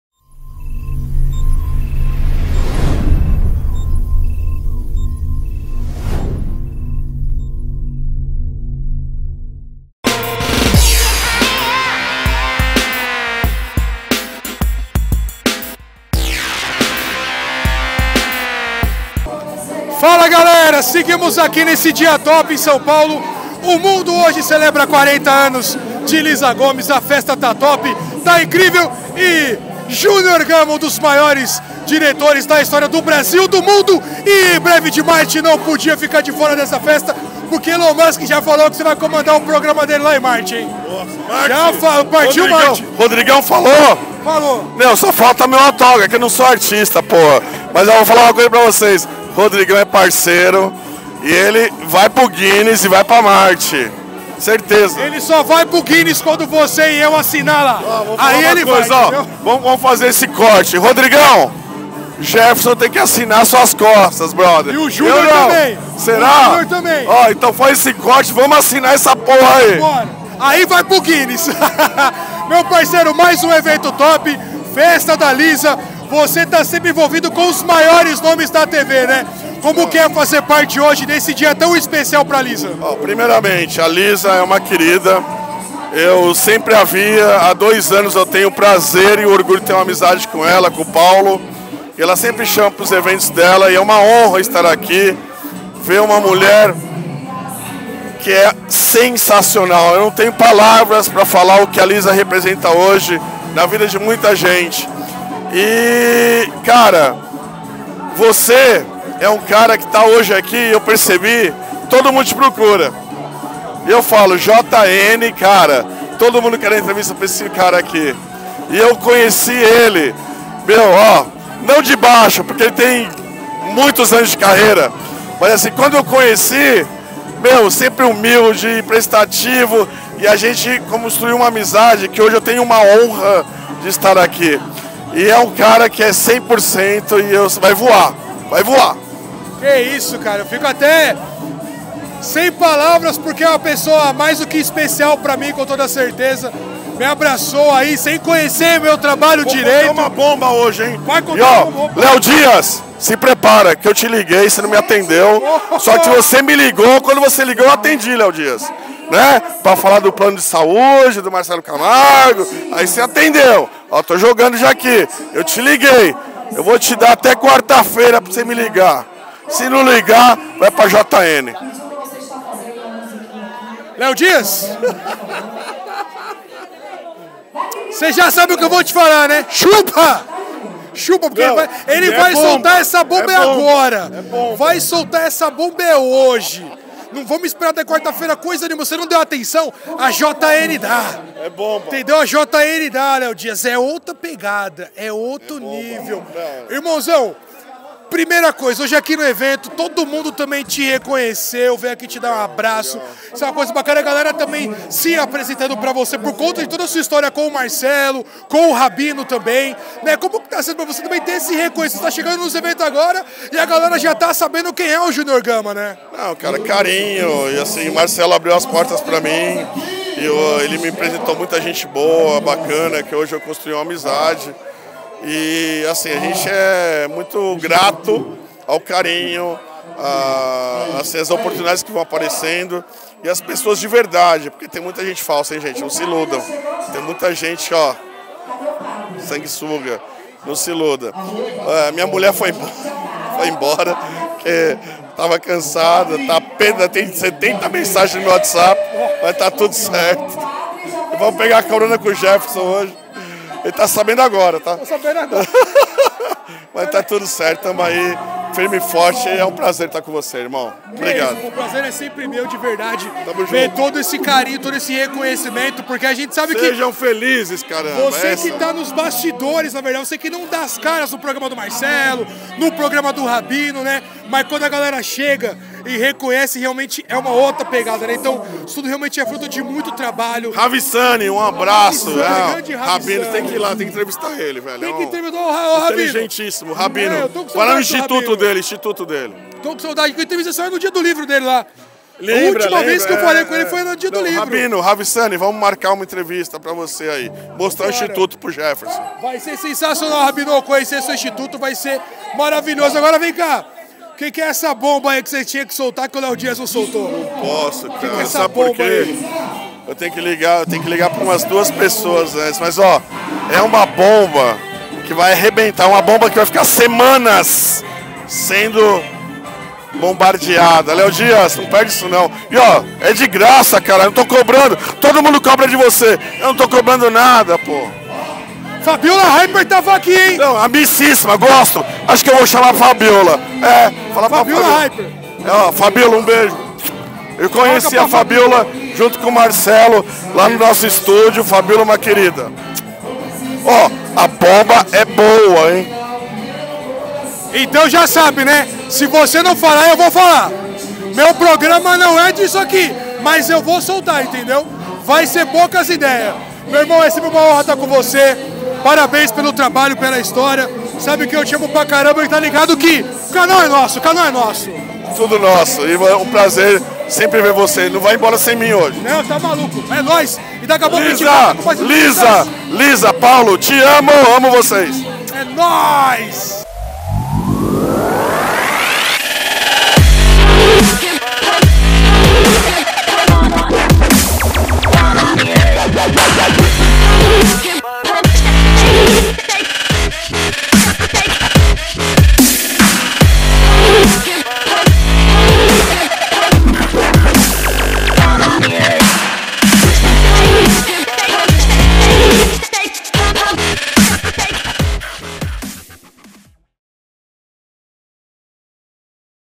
Os Papa Famoso "Aniversário Lisa Gomes" - Entrevista com Júnior Gama
Neste vídeo, vocês acompanham tudo que rolou na festa de aniversário de 40 anos da Jornalista Lisa Gomes e uma entrevista exclusiva com o Diretor Júnior Gama!!!